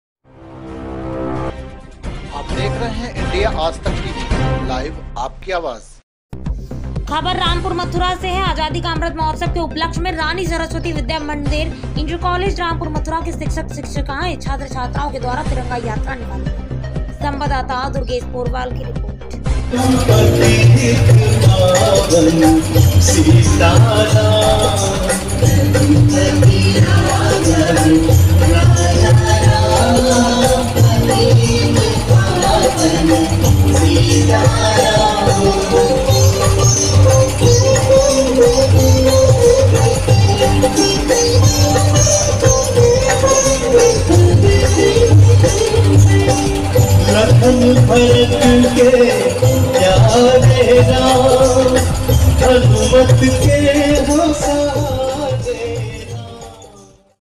रानी सरस्वती विद्या मंदिर इंटर कालेज रामपुर मथुरा के शिक्षक शिक्षिकाएं छात्र-छात्राओं के द्वारा
रामपुर मथुरा।
 आजादी का अमृत महोत्सव के उपलक्ष में रानी सरस्वती विद्या मंदिर इंटर कालेज रामपुर मथुरा के शिक्षक शिक्षिकाएं छात्र-छात्राओं के द्वारा तिरंगा यात्रा निकाली गई जो मुख्य मार्केट रामपुर मथुरा,थाना क्षेत्र,ब्लाक सामने, समेत विभिन्न स्थानों से यात्रा निकालते हुए पुनः विद्यालय परिसर आकर भारत माता की जय, वंदे मातरम के नारों के साथ समापन किया गया।
तिरंगा यात्रा में प्रधानाचार्य मनोज कुमार सिंह, विद्यालय के सभी शिक्षक शिक्षिकाएं, भाजपा मंडल अध्यक्ष मनोज सिंह, विधायक जी के छोटे भाई गुड्डू तिवारी जी,भाजपा प्रतिनिधि लक्ष्मी नारायण मौर्य, युवा मोर्चा मंडल अध्यक्ष धीरेंद्र मिश्रा समेत सैकड़ों की संख्या में लोगों ने भाग लिया। रामपुर मथुरा से दुर्गेश पोरवाल